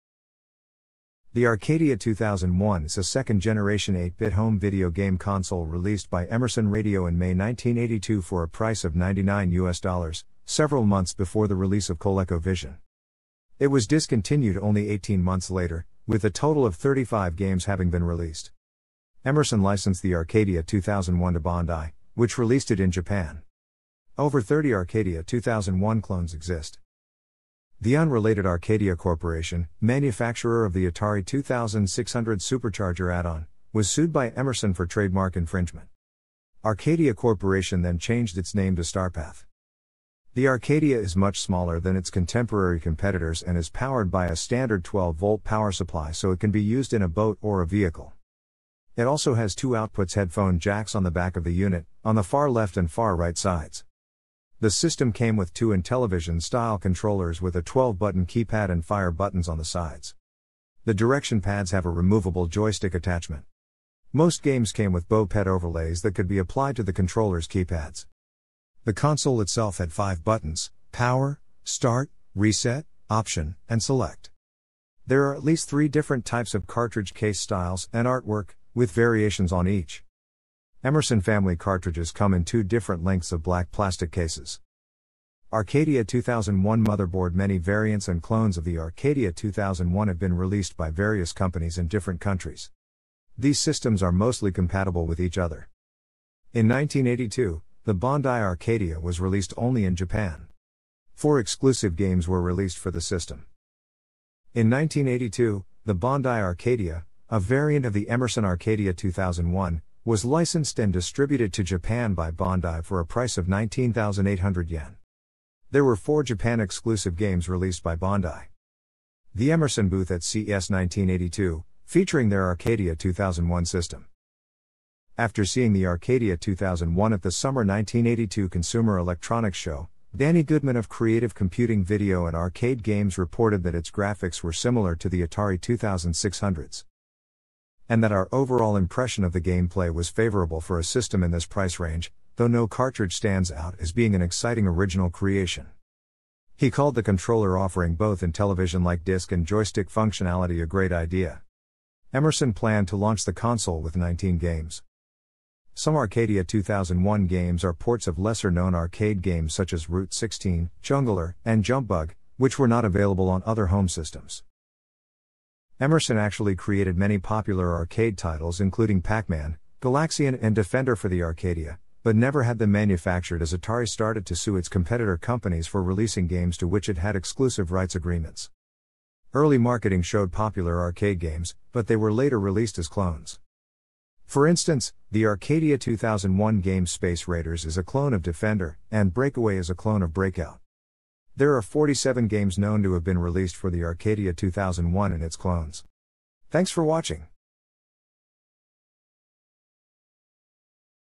Arcadia 2001
Second-generation_video_game_consoles
The Arcadia 2001 is a second-generation 8-bit home video game console released by Emerson Radio in May 1982 for a price of US$ 99, several months before the release of ColecoVision.
It was discontinued only 18 months later, with a total of 35 games having been released.
Emerson licensed the Arcadia 2001 to Bandai, which released it in Japan.
Over 30 Arcadia 2001 clones exist.
The unrelated Arcadia Corporation, manufacturer of the Atari 2600 Supercharger add-on, was sued by Emerson for trademark infringement.
Arcadia Corporation then changed its name to Starpath.
The Arcadia is much smaller than its contemporary competitors and is powered by a standard 12 volt power supply so it can be used in a boat or a vehicle.
It also has two outputs （or inputs） headphone jacks on the back of the unit, on the far left and far right sides.
The system came with two Intellivision-style controllers with a 12-button keypad and 'fire' buttons on the sides.
The direction pads have a removable joystick attachment.
Most games came with BoPET overlays that could be applied to the controller's keypads.
The console itself had five buttons: power, start, reset, option, and select.
There are at least three different types of cartridge case styles and artwork, with variations on each.
Emerson-family cartridges come in two different lengths （short and long） of black plastic cases.
Arcadia 2001 motherboard Many variants and clones of the Arcadia 2001 have been released by various companies in different countries.
These systems are mostly compatible with each other.
In 1982, the Bandai Arcadia was released only in Japan.
Four exclusive games were released for the system.
In 1982, the Bandai Arcadia, a variant of the Emerson Arcadia 2001, was licensed ...